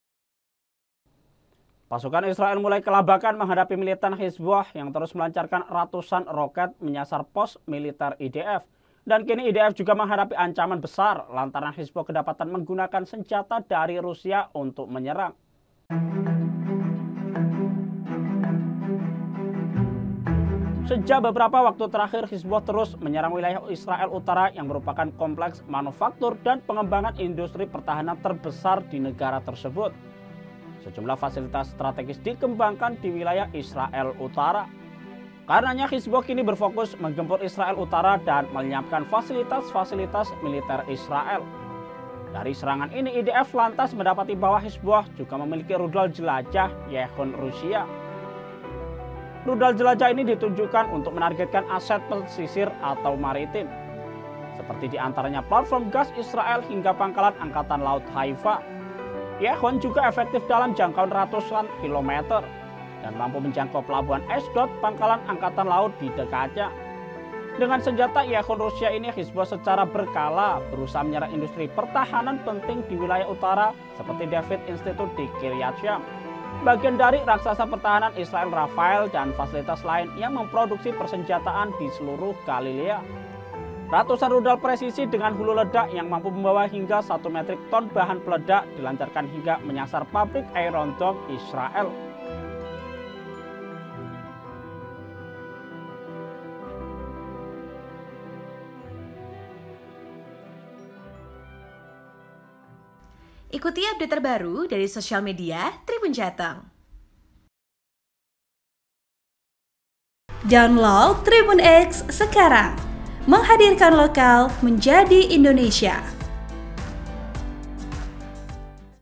IDF Ketar ketir, Hizbullah Pakai Rudal Yakhont Rusia Sasar Pusat Industri Militer Israel
Pasukan Israel mulai kelabakan menghadapi militan Hizbullah yang terus melancarkan ratusan roket menyasar pos militer IDF.

Kini, IDF juga menghadapi ancaman besar lantaran Hizbullah kedapatan menggunakan senjata dari Rusia untuk menyerang.

Sejak beberapa waktu terakhir, Hizbullah terus menyerang wilayah Israel Utara yang merupakan kompleks manufaktur dan pengembangan industri pertahanan terbesar di negara tersebut.

Sejumlah fasilitas strategis dikembangkan di wilayah Israel Utara.

Karenanya, Hizbullah kini berfokus menggempur Israel Utara dan melenyapkan fasilitas-fasilitas militer Israel.

News Anchor : Muhammad Khoiru Anas

Jangan sampai ketinggalan informasi terupdate dan berita pilihan Tribun Jateng. Ikuti saluran kami di WhatsApp Channel